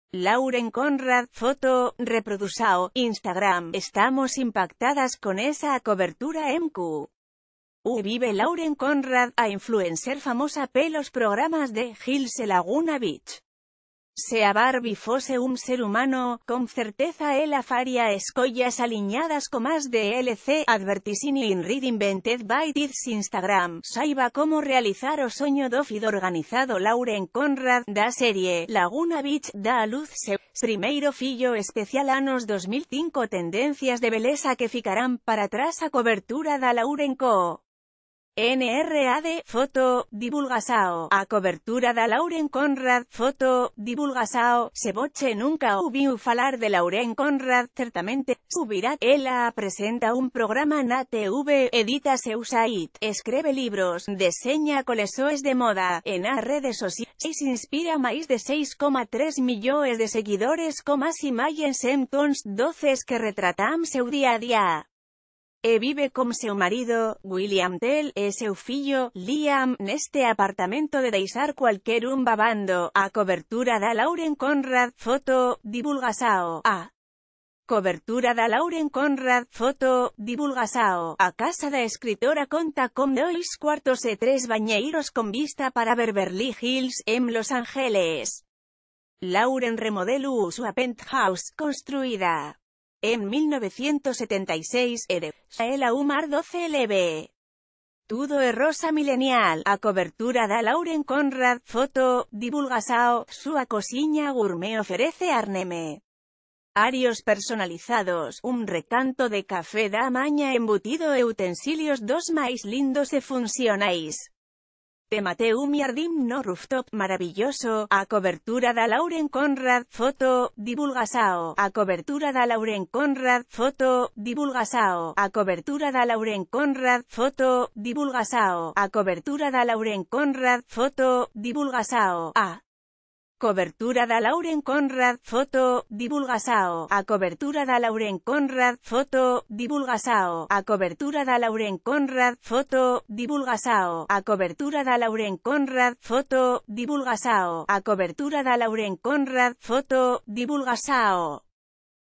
[ENTERTAINMENT NEWS]Casa da Barbie: conheça a cobertura de Lauren Conrad em Los Angeles
Lauren Conrad (Foto: Reprodução/Instagram)Estamos impactadas! Conheça a cobertura em que vive Lauren Conrad, a influencer famosa pelos programas The H...